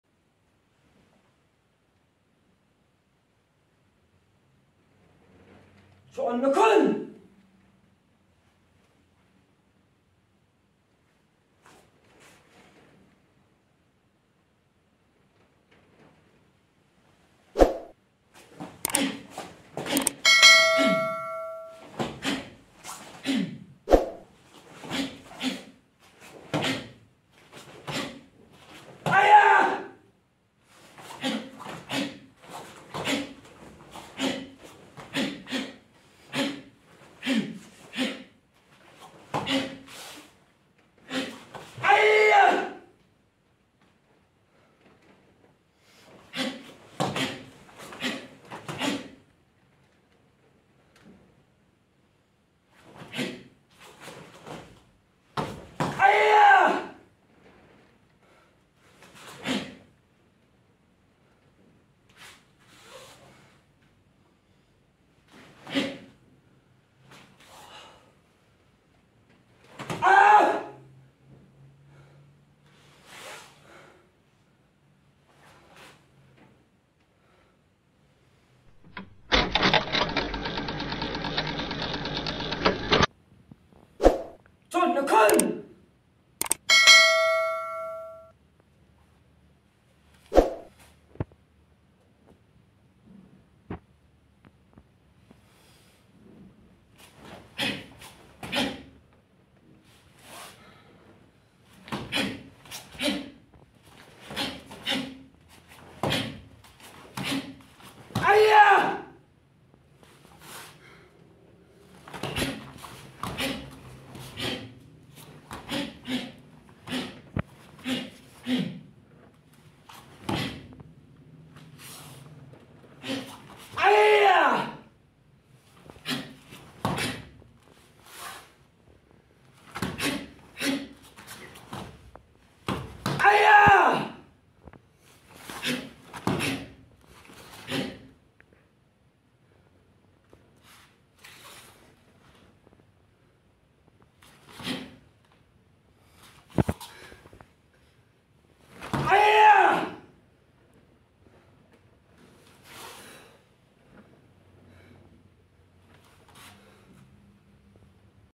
کاتا چو اون نو کون کوبودو kobudo kata cho un no kun
کاتای چو اون نو کُن کوبودو کاراته در دو زاویه مختلف
اجرا توسط خودم زانا ولدبیگی و تقدیم به همه شما عزیزان و استاد عزیزم سنسی عبدالرضا عرفانی بزرگوار
کوبودو دم است و کاراته بازدم ،مکمل هم هستند و بدون هم هیچ هستند
اوس